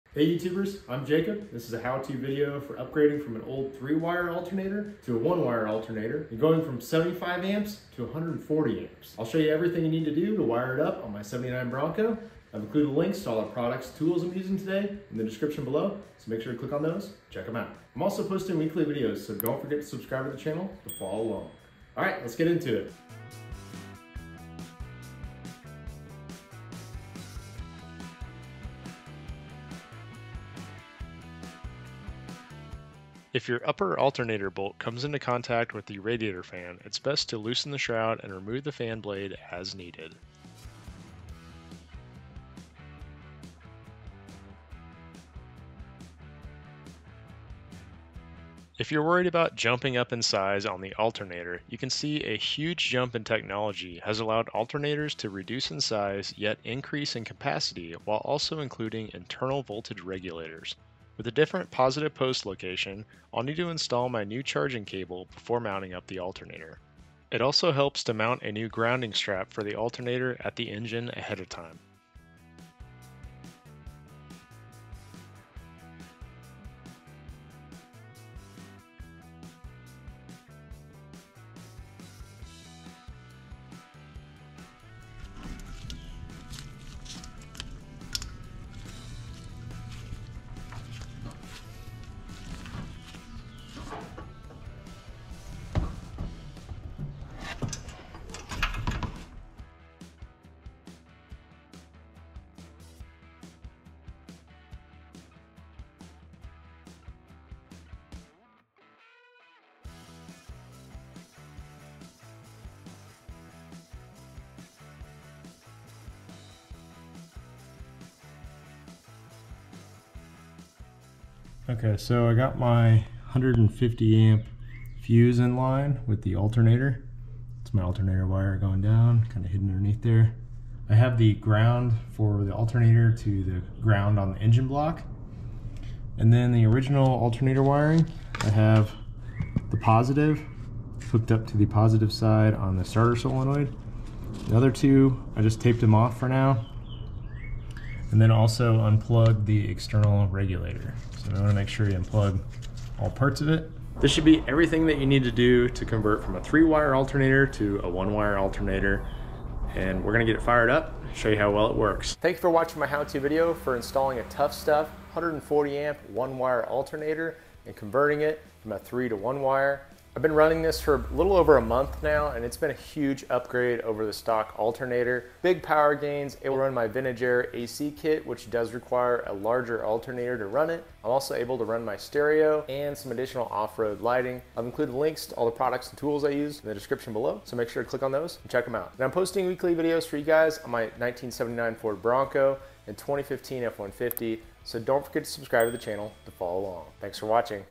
Is a One-Wire Alternator Upgrade Worth It?
- Tuff Stuff 140amp 1 Wire Alternator
- 150amp Mega Fuse
- 4gauge red wiring
- 4awg black wiring
- 4awg wiring terminals W/ heat shrink
- Rechargeable Work light bar
- Multi-tool, Wire Stripper, Cutter and Crimper
- E-Tek Butane Torch
- Dewalt 20v Max Cordless Drill Combo Kit
- Dewalt Mechanics Tool Socket Set
- Ratcheting Wrench set / flex head SAE
- Nitrile Black Gloves
- Southwire Multimeter Test Kit

This is a how to video for upgrading from a 3 wire 75amp externally regulated alternator to a 1 wire 140amp Tuff Stuff internally regulated alternator on 1979 Ford Bronco with a 400cubic inch V8. This applies to all older Ford vehicles equipped with an externally regulated alternator.

If you have a product you would like for me to  review, please reach out.